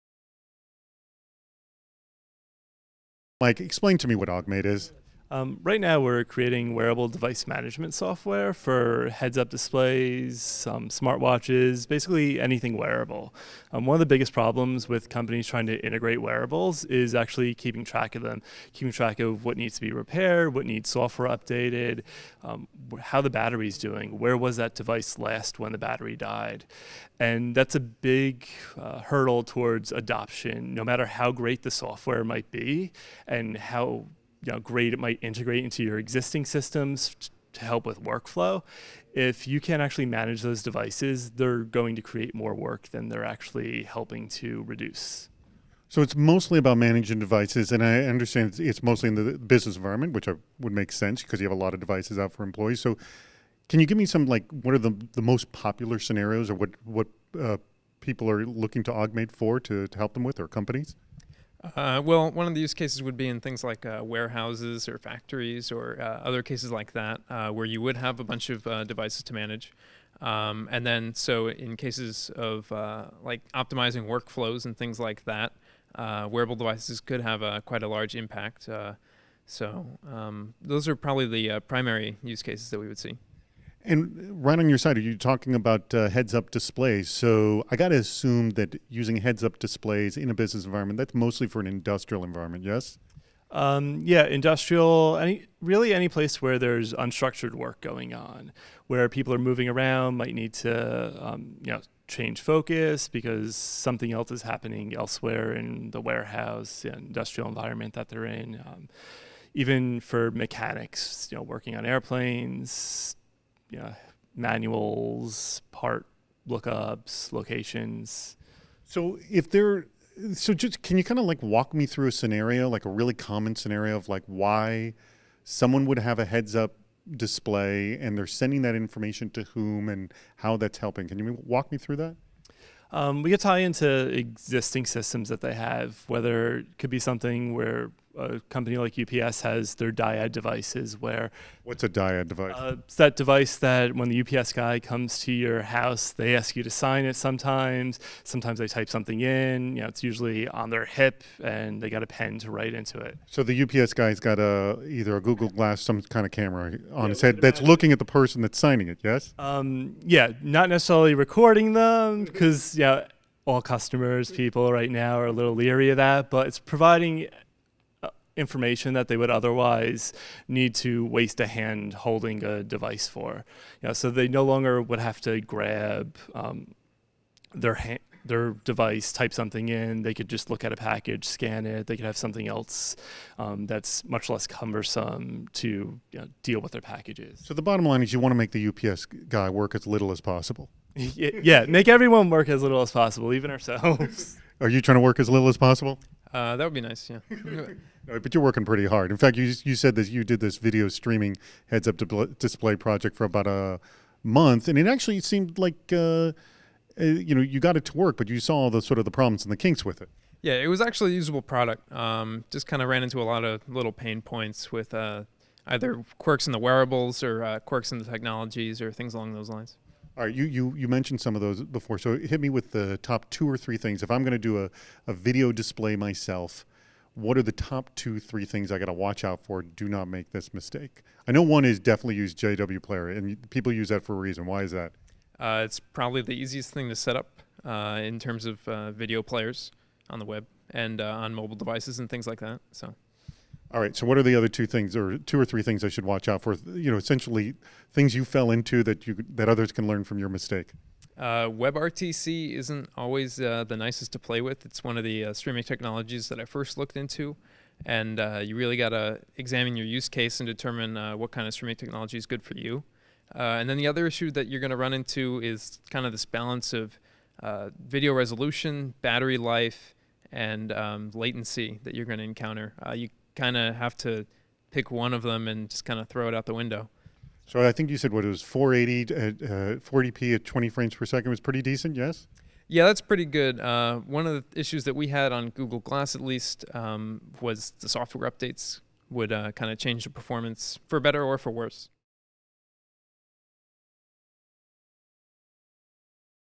Lack of Managing Wearable Devices Could Kill Adoption
Wearable devices provide a lot of wonderful information, especially to improve the productivity of a distributed non-controlled environment. But there’s a huge hurdle towards adoption and that’s maintenance. What devices needs repairs, software updates, and recharged batteries?

“If you can’t manage those devices they’ll create more work than they’re helping to reduce,” admitted Mike DiGiovanni of Augmate, a company that manages wearable devices at companies.

We spoke with DiGiovanni and his colleague Victor Kaiser-Pendergrast at the WearablesTechCon about how their companies manage devices.

These devices work well wherever there’s any unstructured work going on such as at warehouses, factories, or people working in the field. For example, instead of pulling out a scanning device, the UPS guy can just look at a package and scan it.

Streaming video from a head mounted device

We also spoke with Pendergrast about his two-month project to do video streaming from a heads up device. He said from his experience you’re going to have to make a compromise of video resolution, battery life, and latency. You essentially can only have one. Pendergrast said a good sweet spot for video quality was 480p and 20 frames per second.